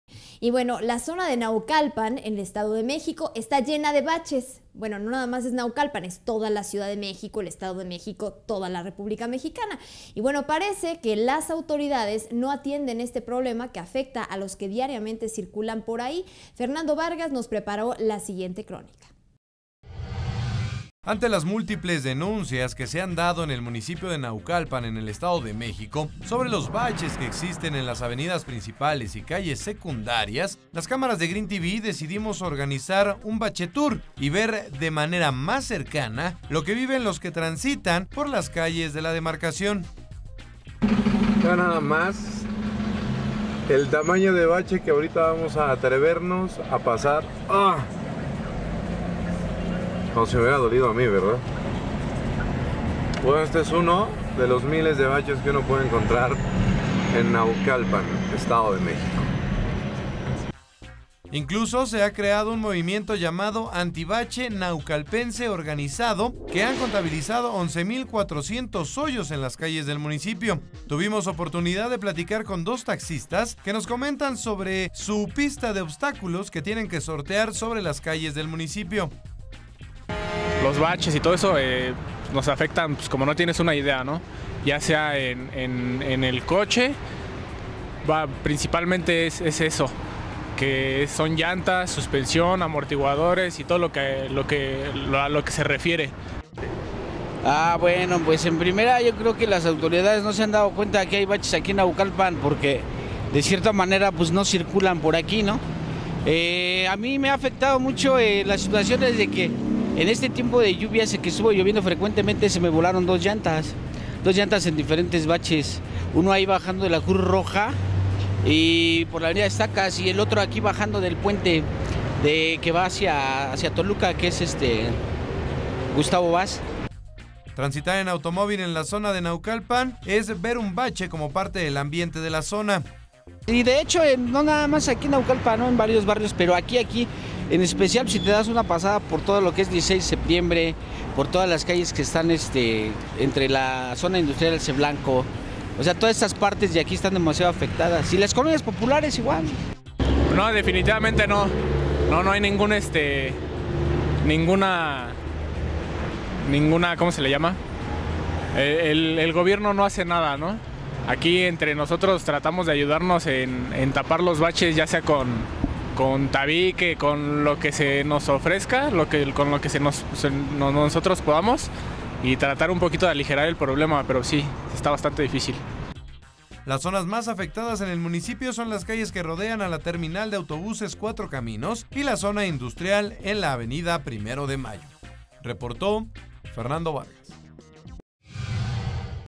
Naucalpan, la ciudad de los baches
La zona de Naucalpan, en el Estado de México, está llena de baches y parece que las autoridades no atienden este problema que afecta a los que diariamente circulan por ahí.

Noticiario Esfera Azul
Producción Greentv, La Televisión Viva